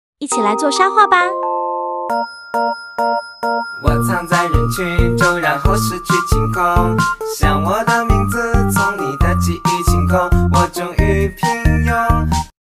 educational toys manual diy creative art
Let's do sand painting together! Exercise children's color matching ability and concentration, stimulate imagination, and allow them to play quietly all afternoon.